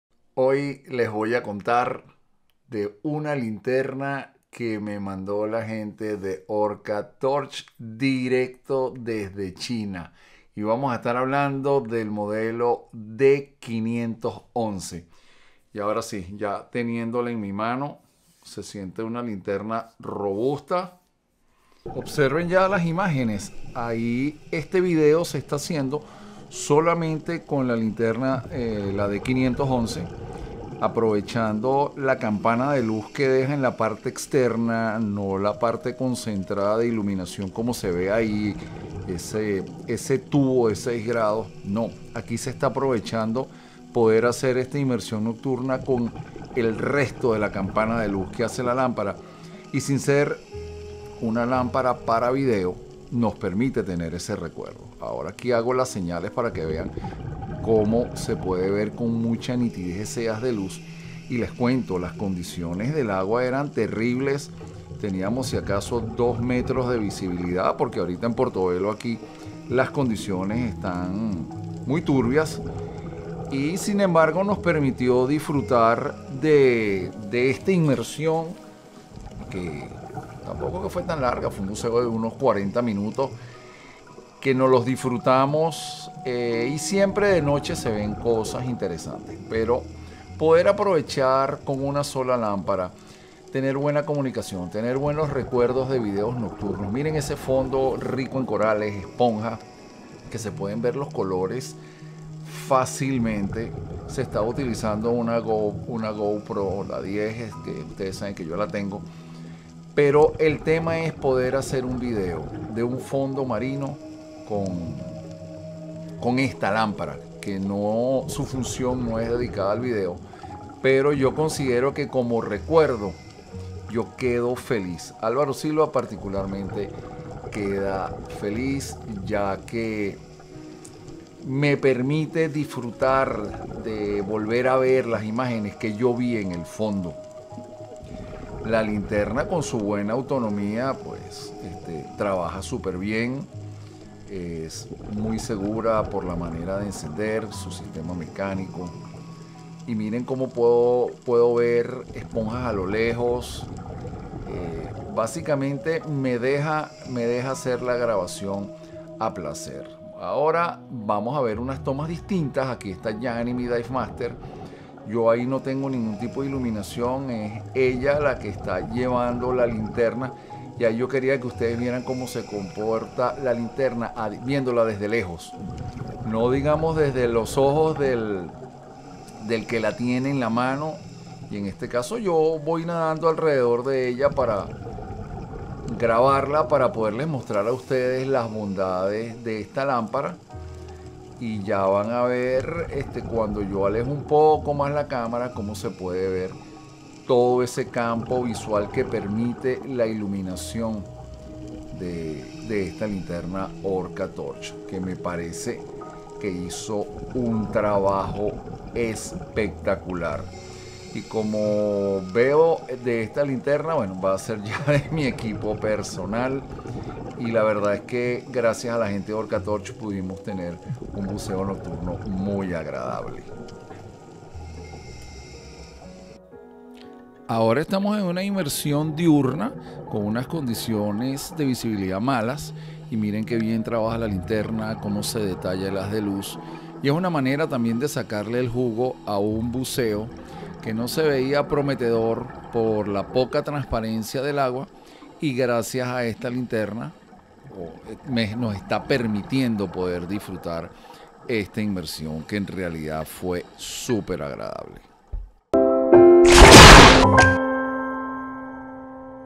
OrcaTorch D511 Technical Dive Light Review | Spanish
Illuminate the underwater world with a narrow spot beam at depths as great as 150m with the OrcaTorch D511 2200 lumens dive torch. The mechanical rotary head switch offers high reliability underwater. The light features a narrow, concentrated 8° spot beam, making it a great tool for focusing, exploring tight spaces, and examining sea life up close.

Video from Alvaro Silva